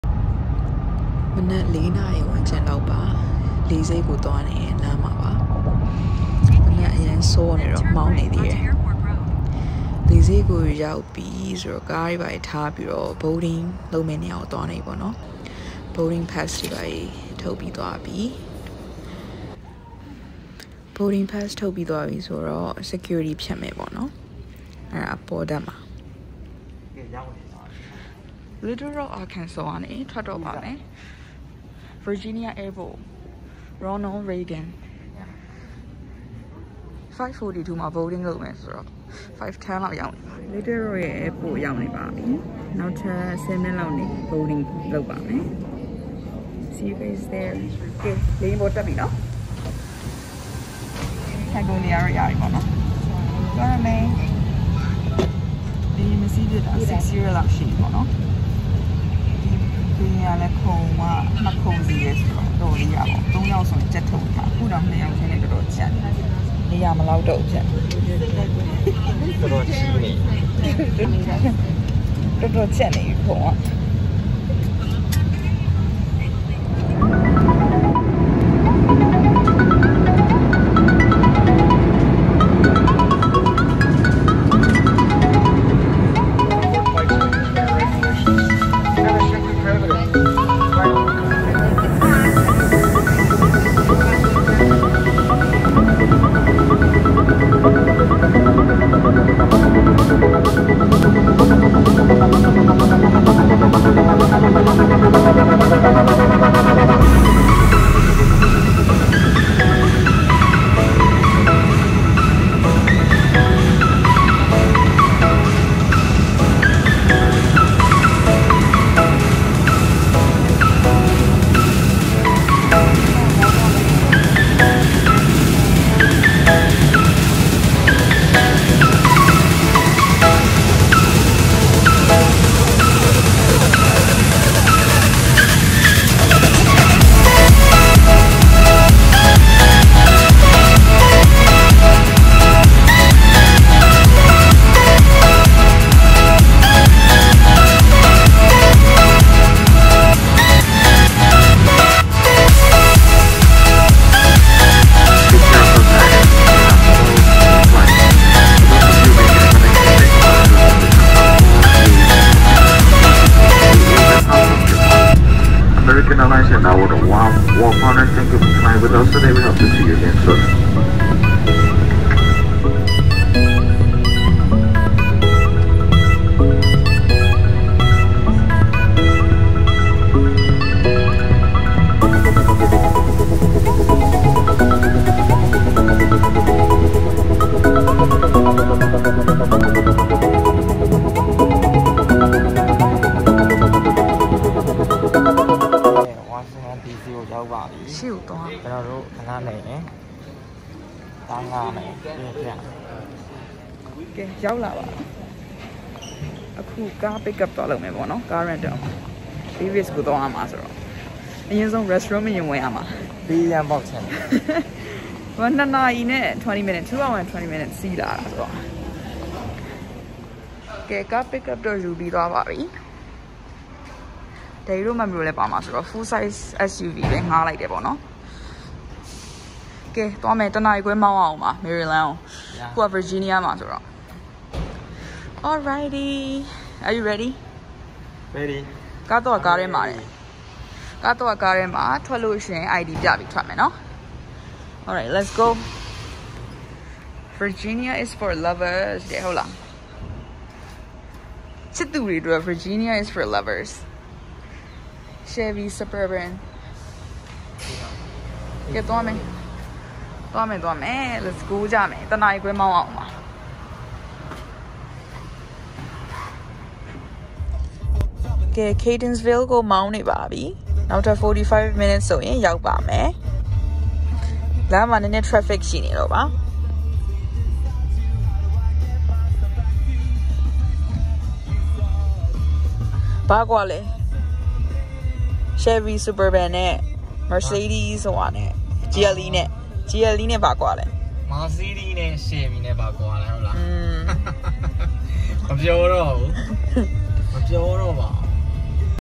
Fly with me ✈️ Little Rock မှ Arlington သို့ခရီးစဉ်✈️
We were flying from Little Rock to Arlington ✈️
Washington,D.C မှာကျင်းပခဲ့တဲ့ ဆောင်းဦးလှိုင်ရန်ပုံငွေဖျော်ဖြေပွဲသွားခဲ့တဲ့ခရီးစဉ်ပါ ☺️